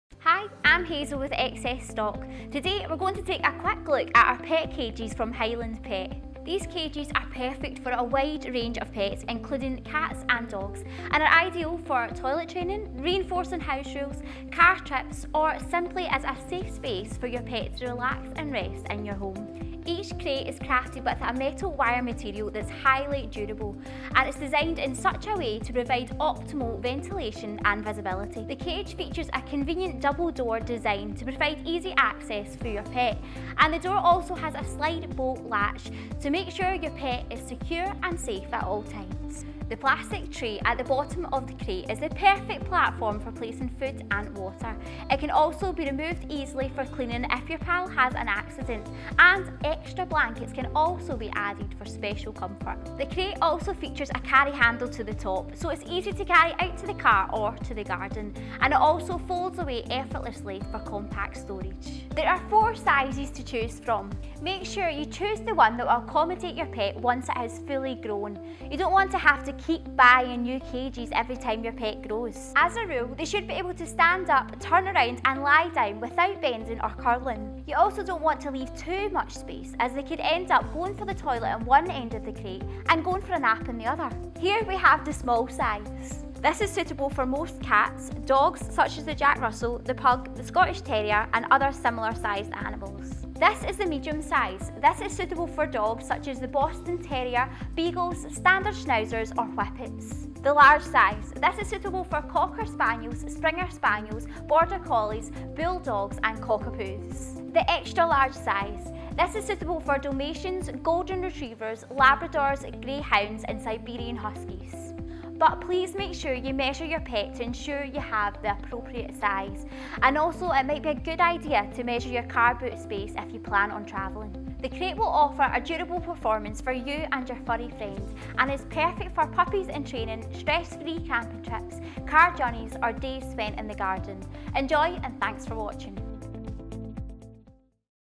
Pet Crate from Highland Pet | Dog & Cat
These Pet Crates from Highland Pet are ideal for a wide range of pets including cats and dogs, and are perfect for toilet training, reinforcing house rules, car trips or simply as a safe space for your pet to rest and relax in your home.

Each crate is crafted with a metal-wire material that's highly durable, and the design ensures your pet receives optimal ventilation and visibility.

The cage features a convenient double door design that provides easy access to your pet and the doors each have a slide bolt latch to ensure your pet remains safe and secure in their new home.

The plastic tray at the bottom of the cage is the perfect platform for placing food and water and it can be removed for easy cleaning if your pal has an accident. You can also add some blankets for extra comfort.

The crate also features a carry handle to the top so it's easy to carry out to the car or the garden, and it folds away effortlessly for compact storage.

There are four sizes to chose from - make sure you choose the one that will accommodate your pet once it has fully grown.
As a rule - he/she should be able to stand up, turn around and lie down without bending or curling.

There are four sizes to choose from;
Small: H51 x D60 x W43cm
Medium: H56 x D76 x W48cm
Large: H65 x D91 x W57cm
Extra Large: H78 x D106.5 x W69.5cm

Please make sure you measure your pet to ensure you have the appropriate size. You might also want to measure your car boot space if you plan on travelling.

The crate will offer a durable performance for you and your furry friend and is perfect for new puppies in training, stress free camping trips, car journeys and days spent in the garden!